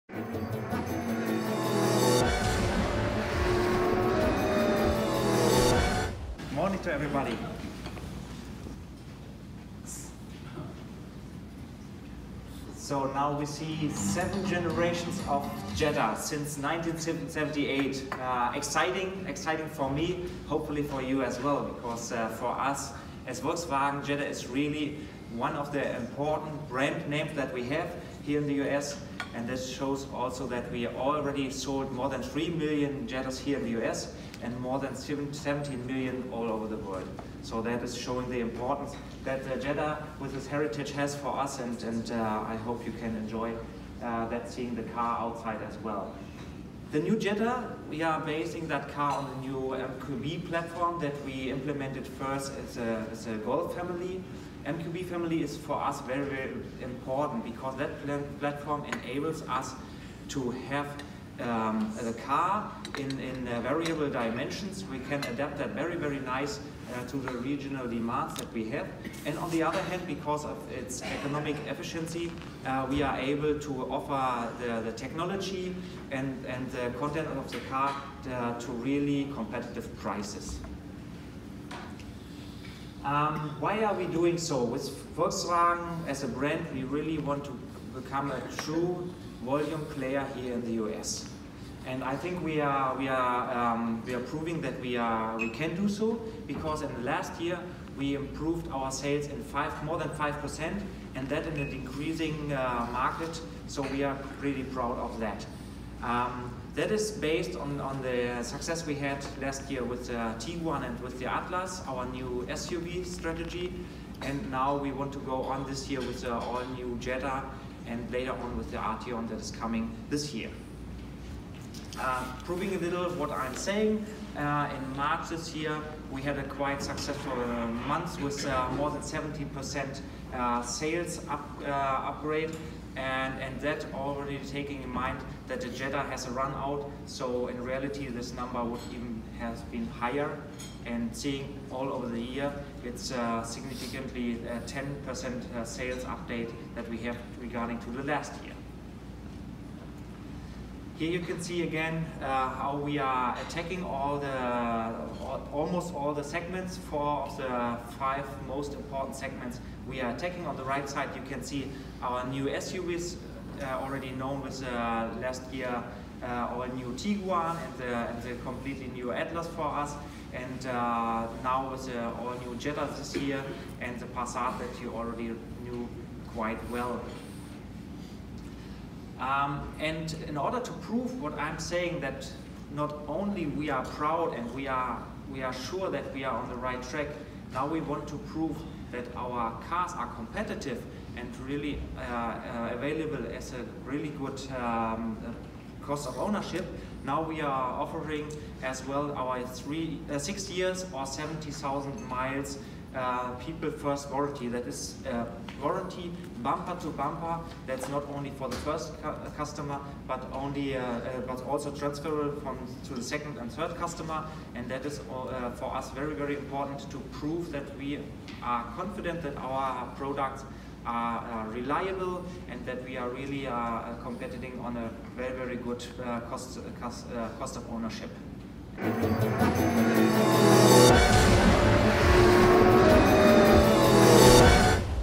2019 Volkswagen Jetta Presentation by Jan Dickmann, VP of Productline VW
2019 Volkswagen Jetta Presentation by Jan Dickmann, VP of Productline VW.

Volkswagen of America, Inc., today announced full pricing of the all-new 2019 Jetta. Now based off the award-winning MQB platform, the 2019 Jetta offers bolder design, more interior space than the outgoing model and new technology.

The new Jetta will continue to be powered by Volkswagen’s 1.4-liter turbocharged and direct-injection TSI® engine, making 147 horsepower and 184 pound-feet of torque. The power is taken to the front wheels via a new standard six-speed manual transmission, with an eight-speed automatic transmission optional on the base model and standard on higher trims. Automatic transmission models come standard with a Start/Stop system designed to improve fuel efficiency. This technology stops the engine during idle when the brake pedal is held (such as when waiting at a stop light). When the brake pedal is released, the engine restarts.

Every Jetta offers standard Driver Personalization with up to four driver settings. Dependent on trim, customizable features include: driver seat memory, drive mode selection, driver assistance system preferences, climate control, Volkswagen Digital Cockpit arrangement, ambient lighting color, radio presets, navigation view, and more.

The technology of 2019 Jetta also includes offers a combination of both passive and active safety systems that are engineered to meet or exceed current crash regulations. These systems include the class exclusive Automatic Post-Collision Braking System.

Every Jetta model will come with the People First Warranty, America’s Best Bumper-To-Bumper New Vehicle Limited Warranty. The warranty provides coverage for six-years or 72,000-miles (whichever occurs first), and the warranty can be transferred to subsequent owners throughout the remainder of its duration.

The 2019 Volkswagen Jetta will be available in S, SE, R-Line®, SEL, and SEL Premium trim levels and is expected to arrive at U.S. Volkswagen dealers in the second quarter of 2018.